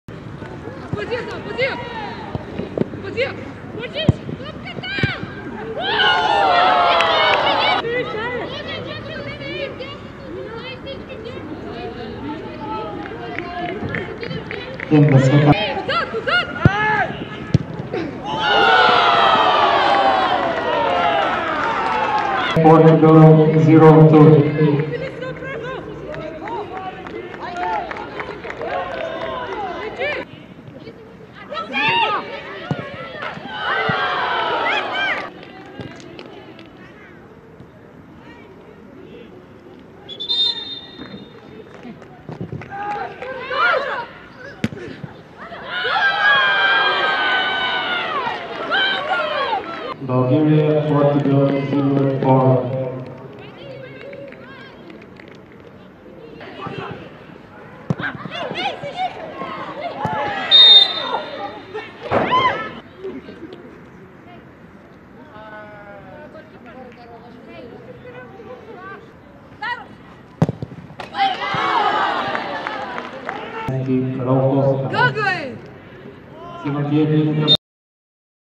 Bulgária 0-5 Portugal (Seleção Feminina)
Missão cumprida! A Seleção Nacional Feminina A garantiu, esta terça-feira, mais três pontos na caminhada rumo ao Campeonato do Mundo de 2023, ao vencer por 5-0 na Bulgária, naquela que foi a sua terceira vitória consecutiva na fase de qualificação.

Consulta a nossa programação em: canal11.pt